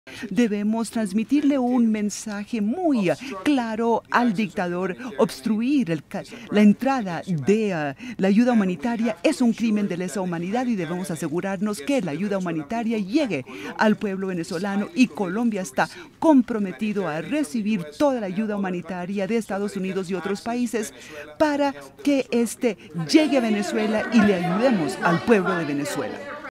Duque advierte a Maduro que bloqueo de ayuda es ‘crimen contra la humanidad’
En el marco de su visita oficial a Estados Unidos, donde a esta hora se reúne con el mandatario de ese país, Donald Trump, el presidente colombiano Iván Duque, le advirtió al gobernante venezolano, Nicolás Maduro, que bloquear el ingreso de ayuda humanitaria a Venezuela será considerado un “crimen contra la humanidad”.